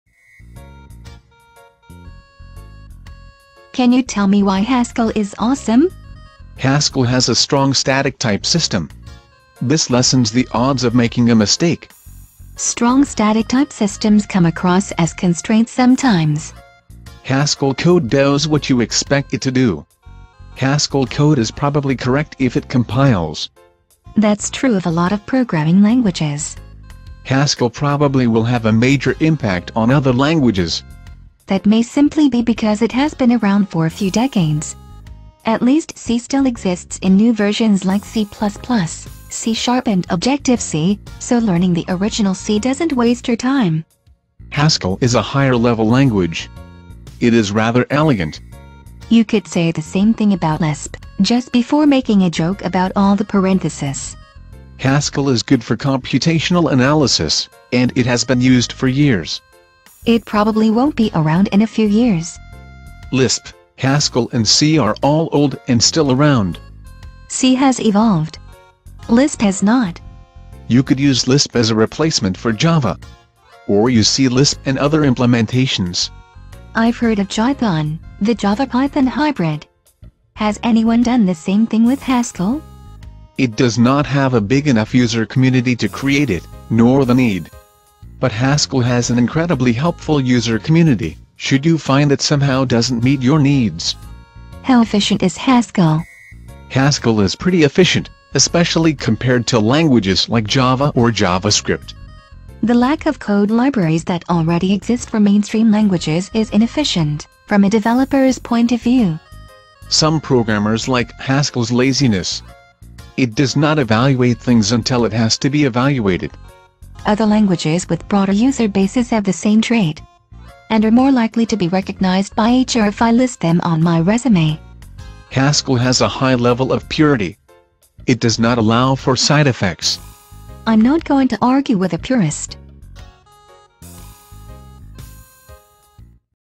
Why Haskell is Awesome
Can you tell me why Haskell is awesome?

Haskell has a strong static type system. This lessens the odds of making a mistake.

Strong static type systems come across as constraints sometimes.

Haskell code does what you expect it to do. Haskell code is probably correct if it compiles.

That's true of a lot of programming languages.

Haskell probably will have a major impact on other languages.

That may simply be because it has been around for a few decades. At least C still exists in new versions like C++, C# and Objective C, so learning the original C doesn't waste your time.

Haskell is a higher level language. It is rather elegant.

You could say the same thing about Lisp, just before making a joke about all the parenthesis.

Haskell is good for computational analysis, and it has been used for years.

It probably won't be around in a few years.

Lisp, Haskell and C are all old and still around.

C has evolved. Lisp has not.

You could use Lisp as a replacement for Java. Or use C-lisp and other implementations.

I've heard of Jython, the Java-Python hybrid. Has anyone done the same thing with Haskell?

It does not have a big enough user community to create it, nor the need. But Haskell has an incredibly helpful user community, should you find it somehow doesn't meet your needs.

How efficient is Haskell?

Haskell is pretty efficient, especially compared to languages like Java or JavaScript.

The lack of code libraries that already exist for mainstream languages is inefficient, from a developer's point of view.

Some programmers like Haskell's laziness. It does not evaluate things until it has to be evaluated.

Other languages with broader user bases have the same trait. And are more likely to be recognized by HR if I list them on my resume.

Haskell has a high level of purity. It does not allow for side-effects.

I'm not going to argue with a purist.